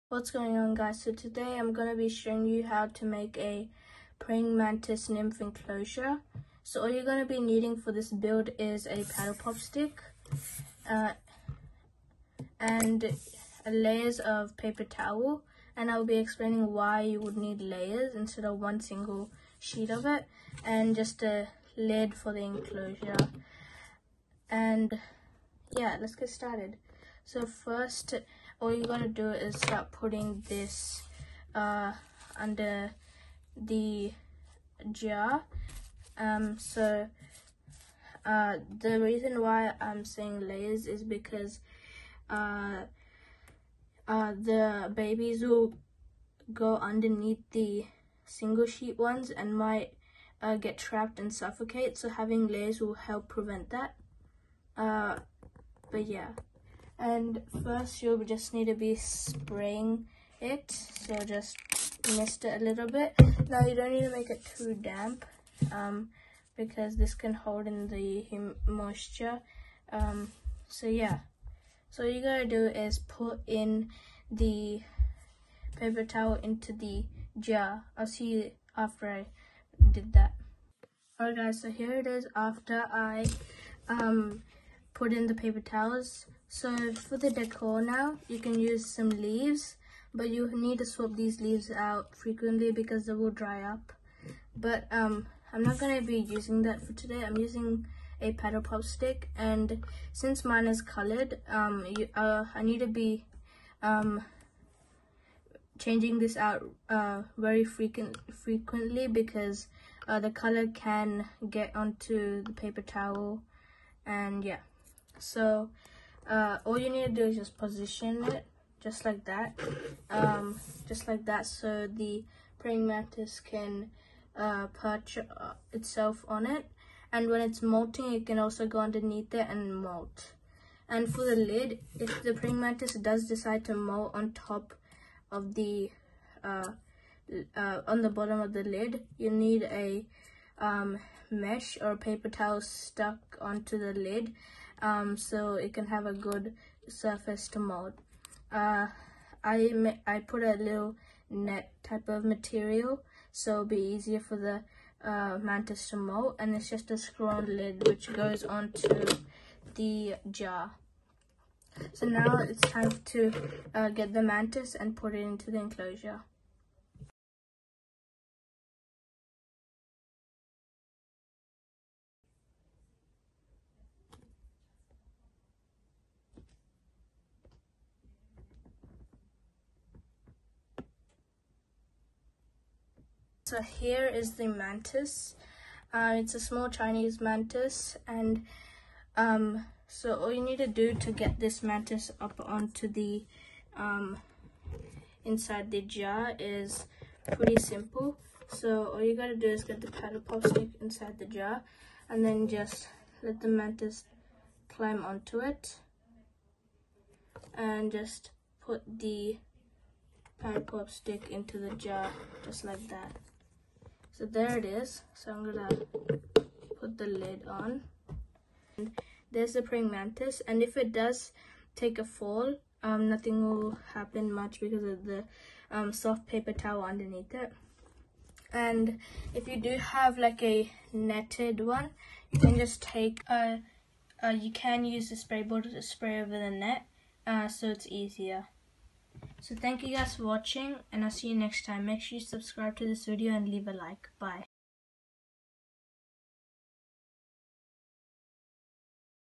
How to make a Praying Mantis nymph Enclosure ( Easy Tutorial)
watch this easy tutorial on how to make a nymph enclosure with some paper towels and a paddle pop stick.